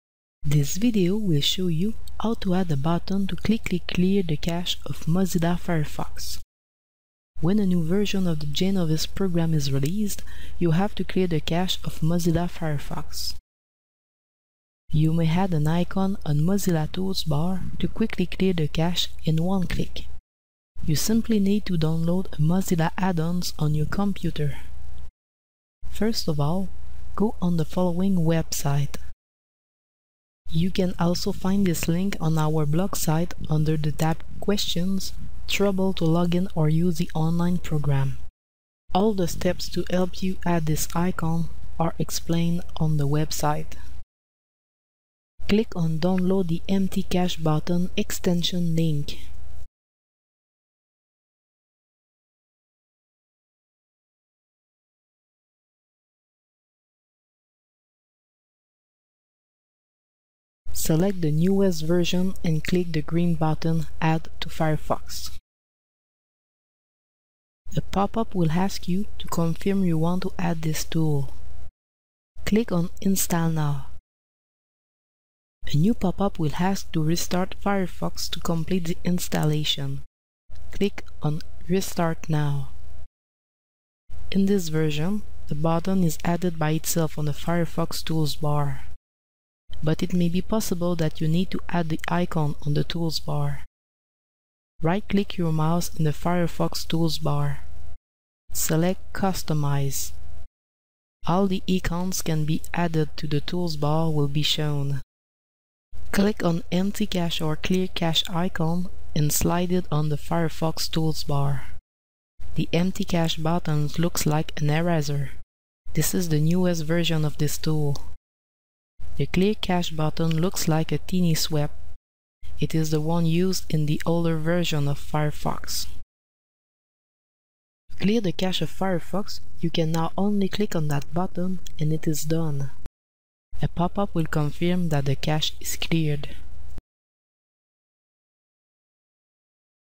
GenOvis - English - Mozilla Firefox - Clear the cache button
This video will show you how to add a clear cache button in Mozilla Firefox toolbar to easily clear the cache of your browser.